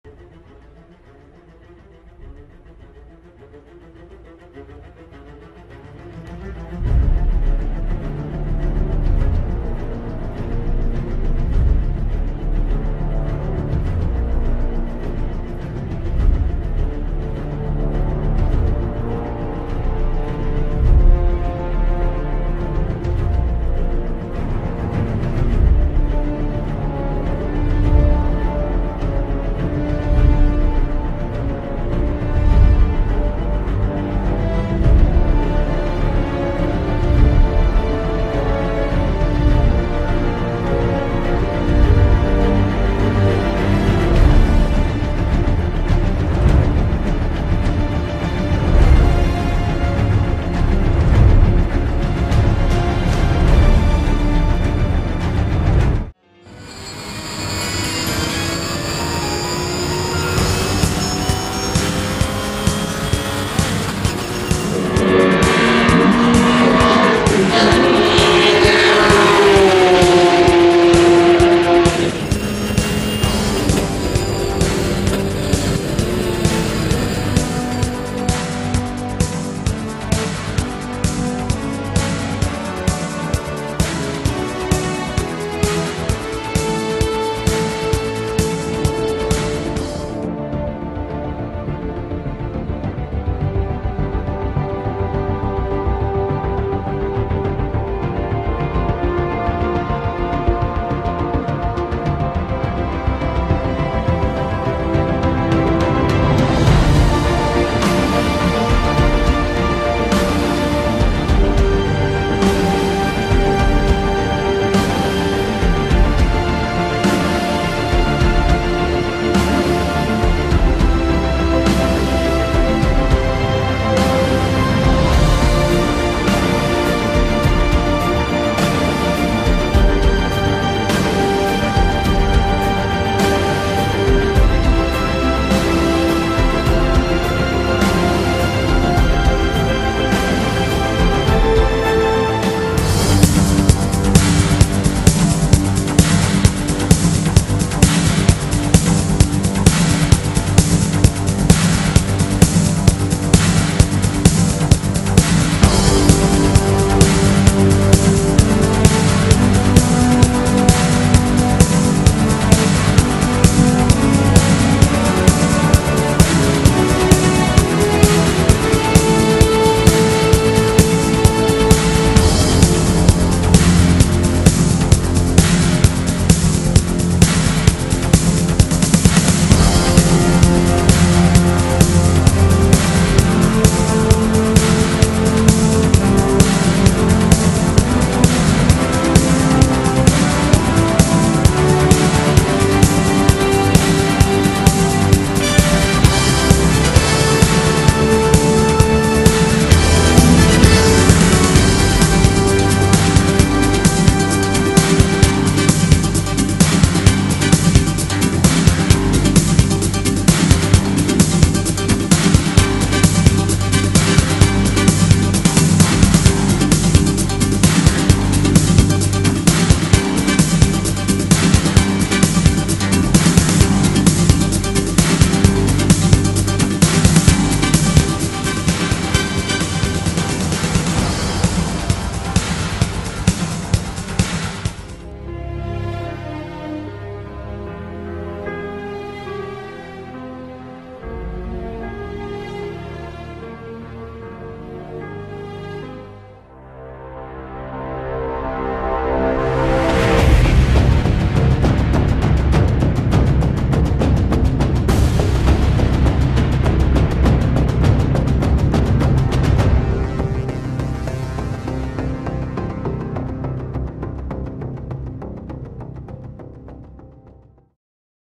Procar Motorsport Racing - Saisonrückblick 2013
Ein beschwingter und emotionaler Saisonrückblich 2013 von Peter Schober und Procar Motorsport. Das Video kann man auch hier downloaden, zum ruckelfreien Betrachten: